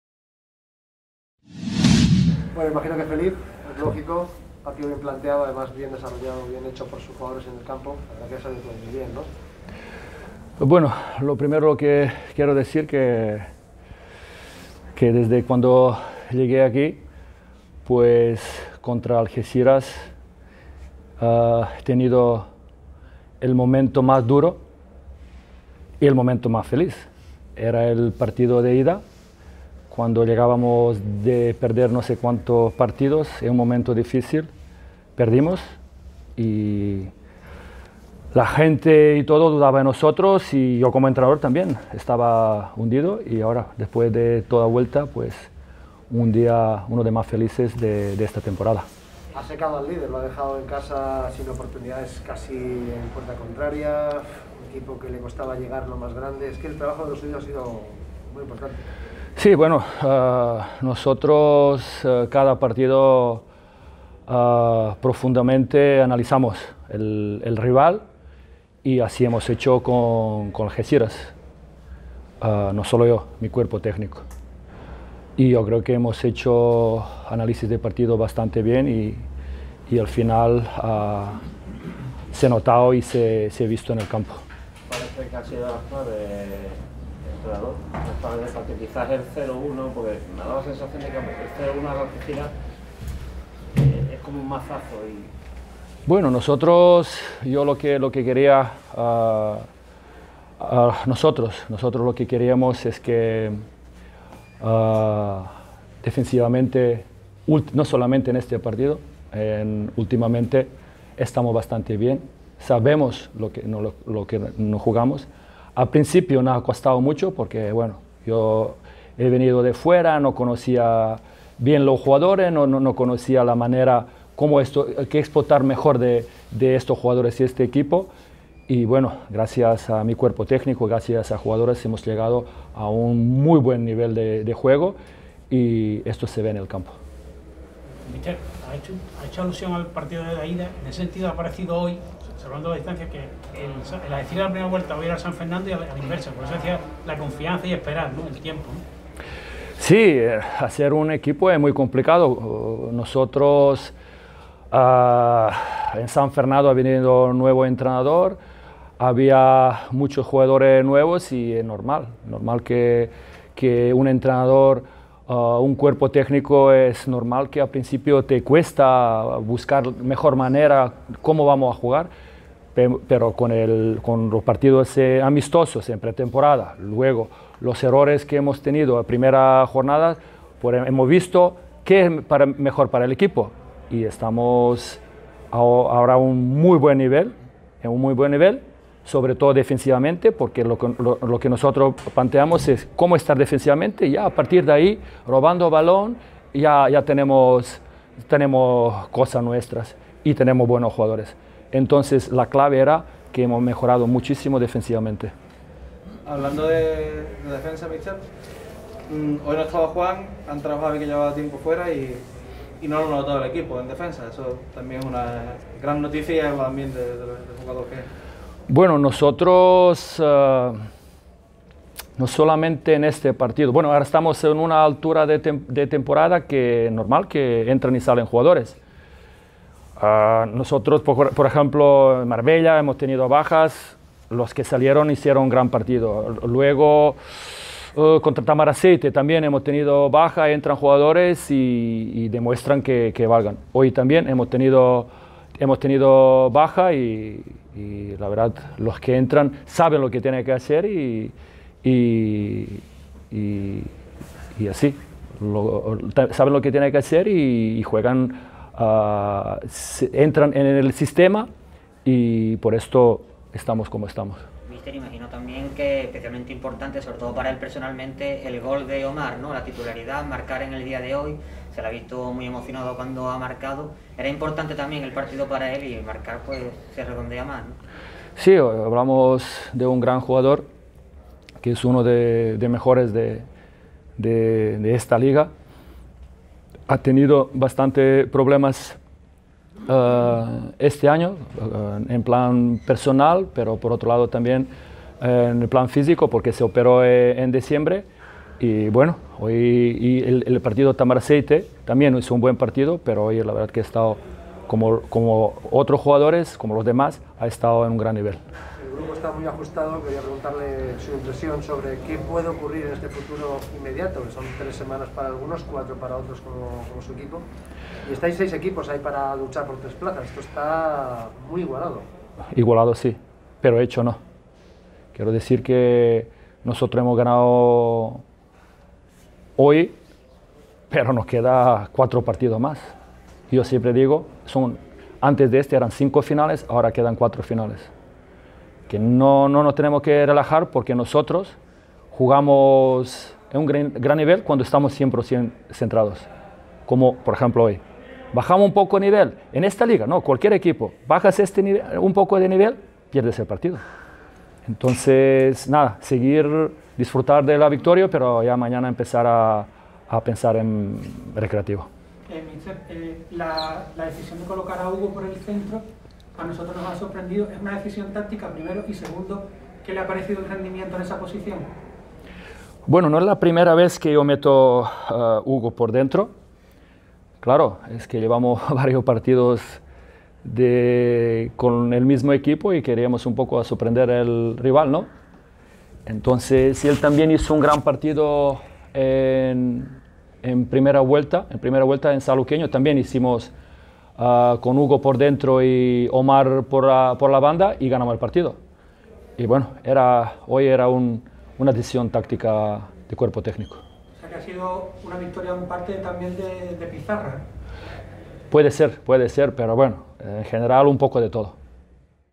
Jovan Stankovic: "Estamos a un muy buen nivel"
Rueda de prensa del entrenador del San Fernando CD, Jovan Stankovic, tras la victoria en Algeciras.